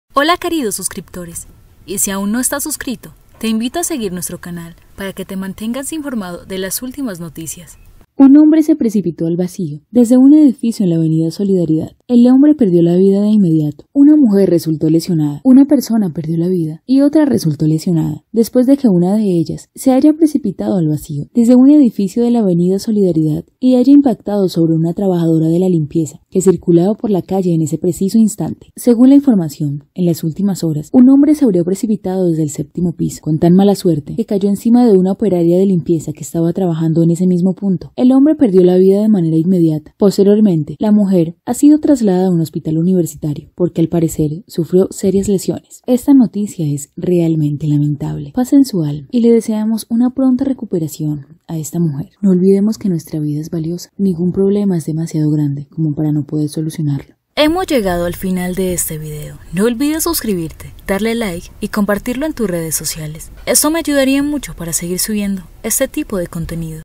🔴¡ ULTIMA HORA HACE UNAS HORAS ! muy malas noticias para colombia LUTO NACIONAL TRISTE NOTICIA
Un hombre cayo al vacio desde un edificio de avenida solidaridad y cae sobre una mujer barrendera
malas noticias hoy, Noticias de Hoy! ACABAN DE CONFIRMAR LA NOTICIA luto nacional, PREOCUPACION EN COLOMBIA - COLOMBIA NOTICIAS DE HOY